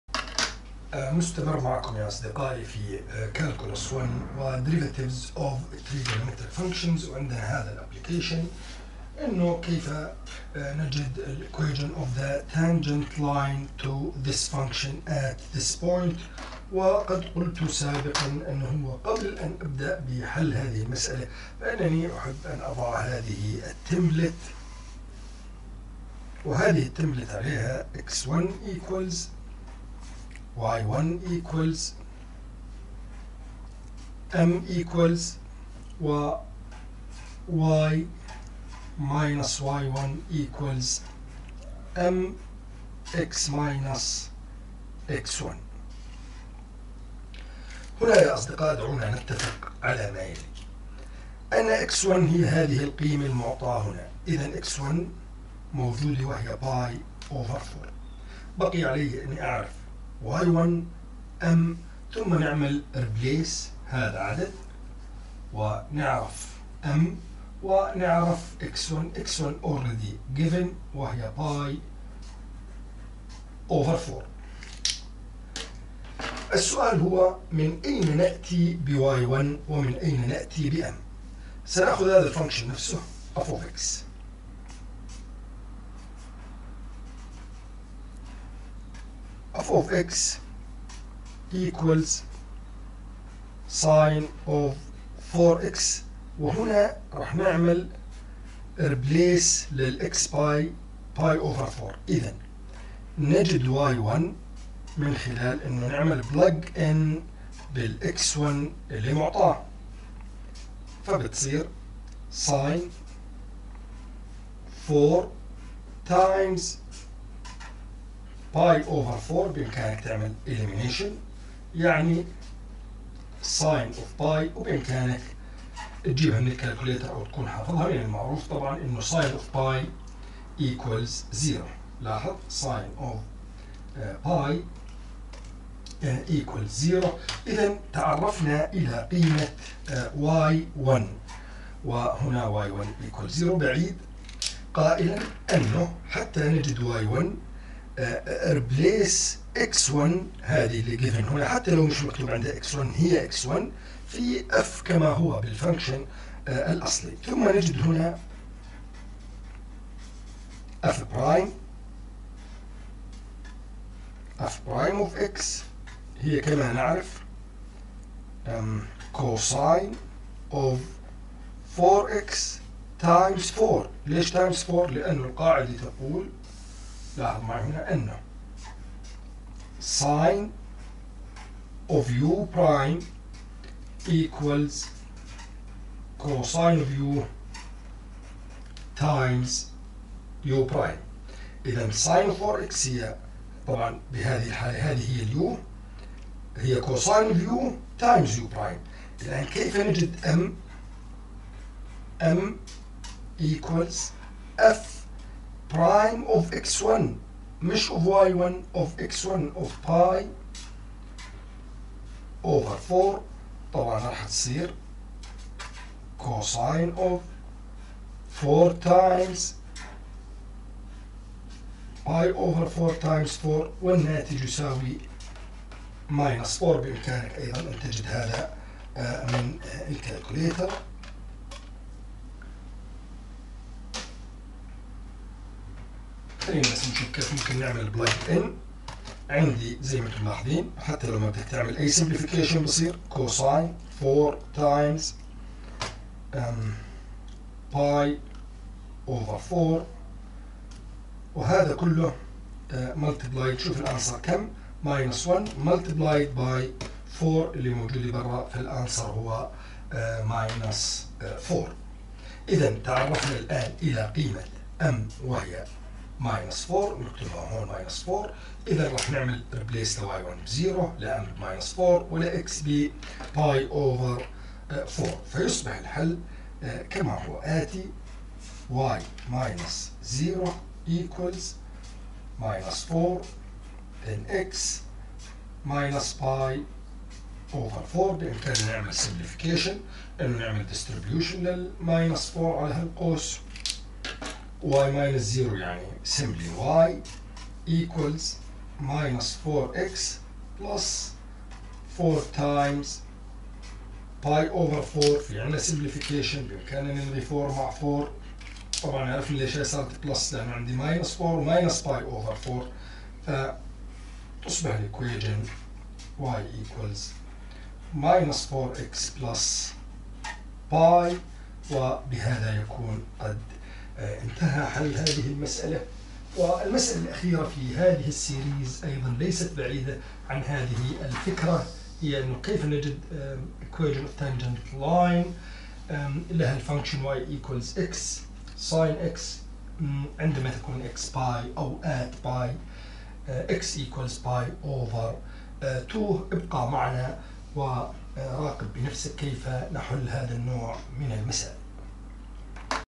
Calculus 1: Derivatives of Trigonometric Functions-13
Calculus 1, the derivatives of trigonometric functions  for university students. The language of explanation is Arabic in addition to English when it is required.